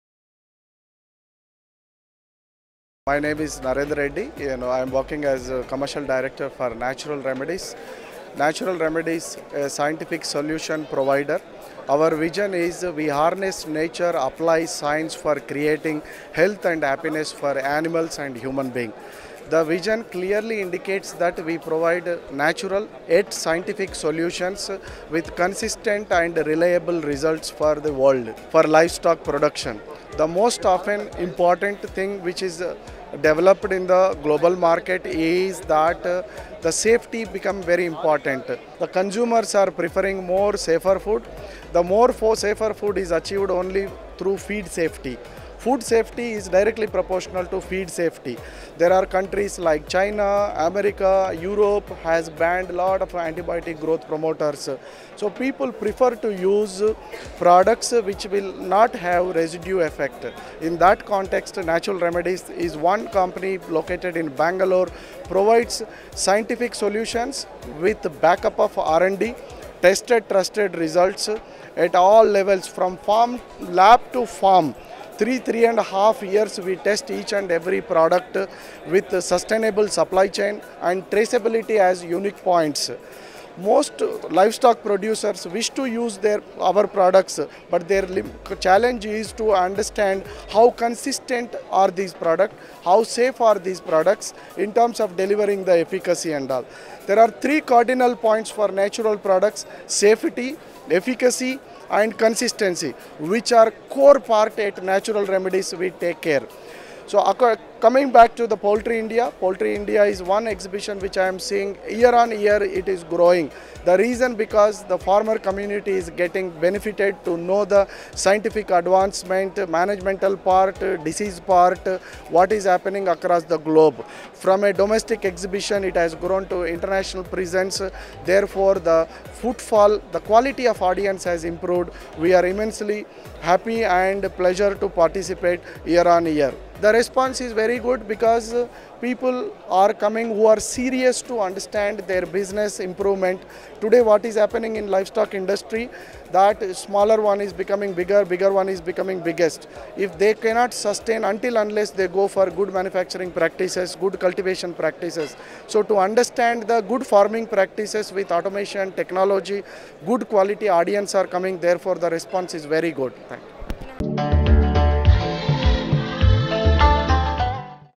Poultry Health Products & Veterinary Wound Care Products - Natural Remedies | Narender Reddy
Narender Reddy, Commercial Director - Natural Remedies | Animal Health Care Products | 13th Edition Poultry India Expo 2019 Hyderabad